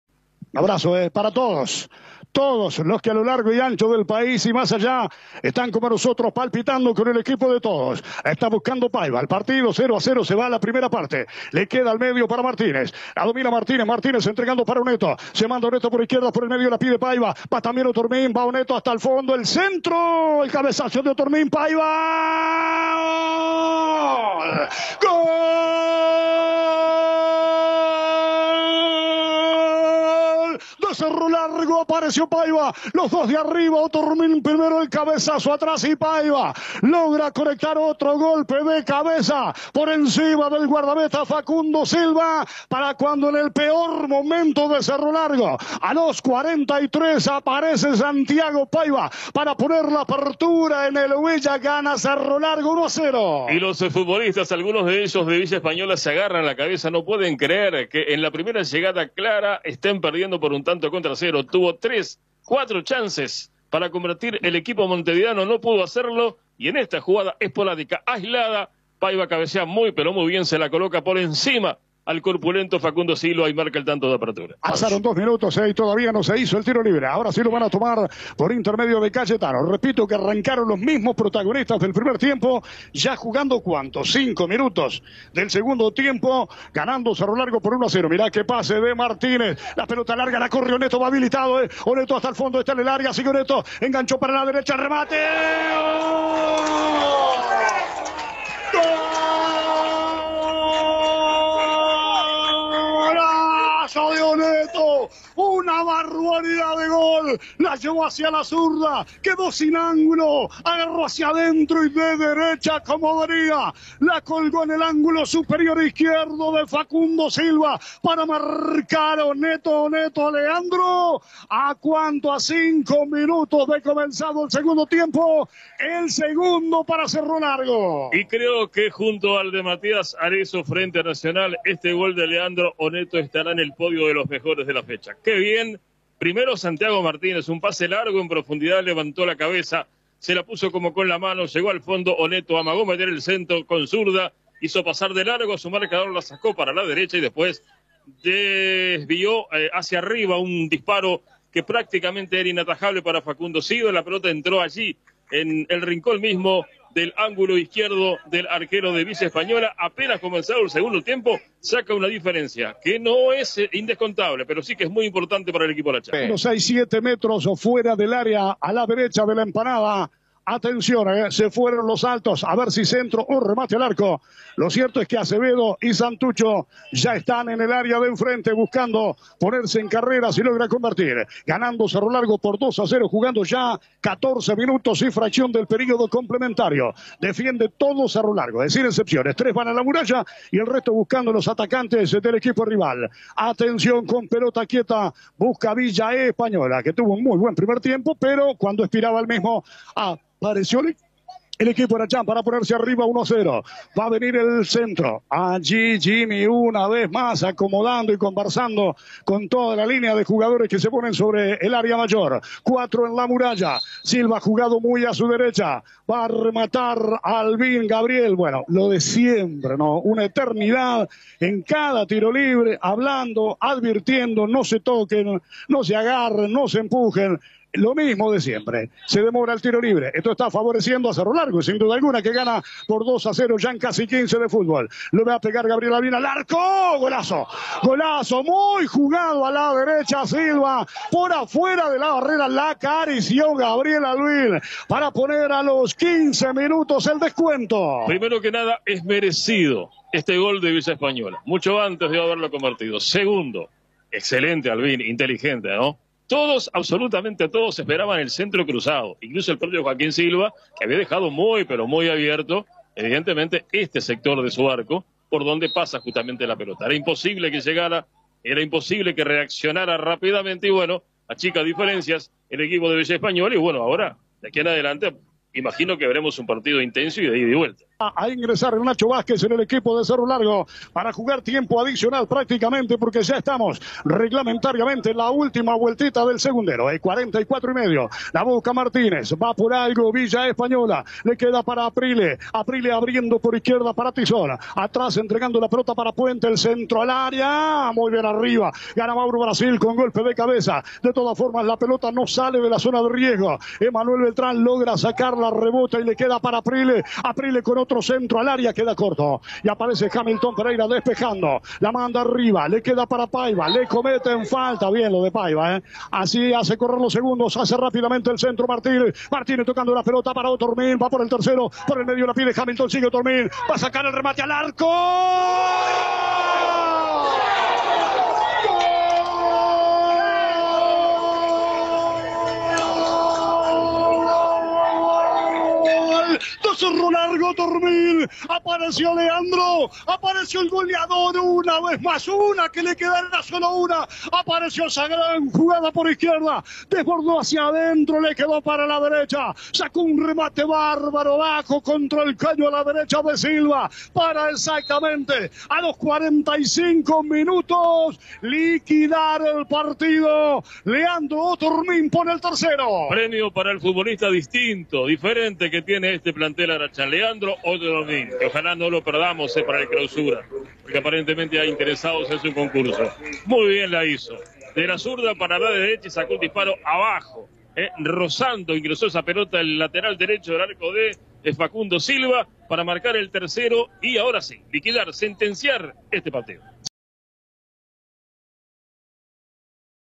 Cerro Largo FC 3 Villa Española 1 - Apertura 2021 (por Valentín Borba Fernández)
Los goles del partido, por Valentín Borba Fernández y Alberto Castro para CW 53 La Voz de Melo 1340 AM.